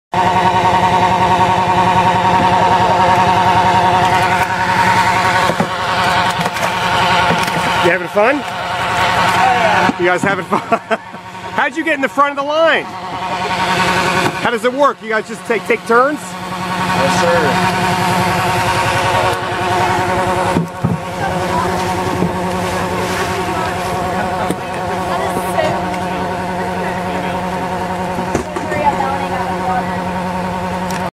Trout or Cheat - Cadets having fun
The Preston County, W.Va. non-profit Trout for Cheat, Inc. is releasing 1,700 pounds of rainbow trout into the Cheat River.  Cadets from the W.Va. Mountaineer ChalleNGe Academy are helping out.